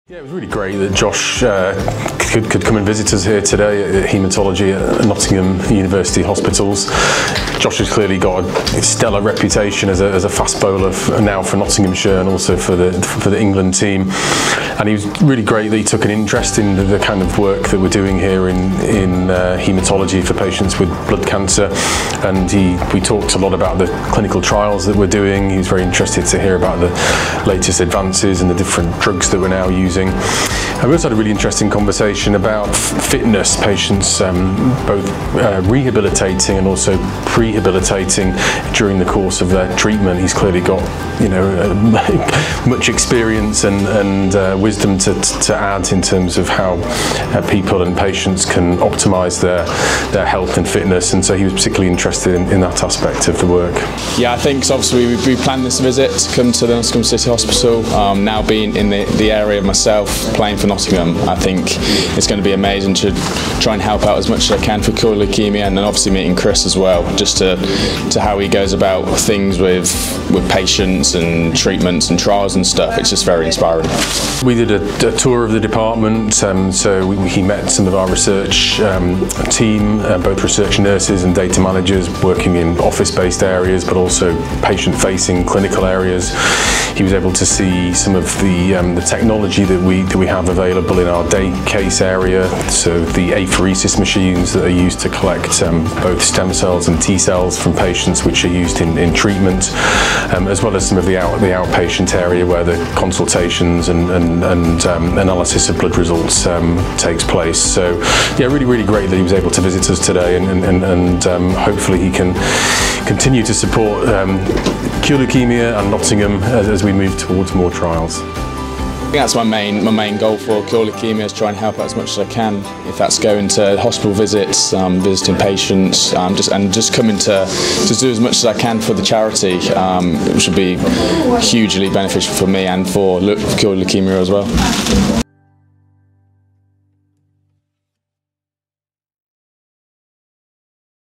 Cure Leukaemia Ambassador and England Cricket Bowler Josh Tongue visits Nottingham TAP Centre
Cure Leukaemia Ambassador and England & Nottinghamshire Fast Bowler Josh Tongue recently visited the charity's Nottingham TAP Centre at Nottingham University Hospitals NHS Trust recently to offer his support to the team and take a look around the excellent facilities on offer to blood cancer patients in the region, whilst lending support to the team taking on the 2024 Paris Marathon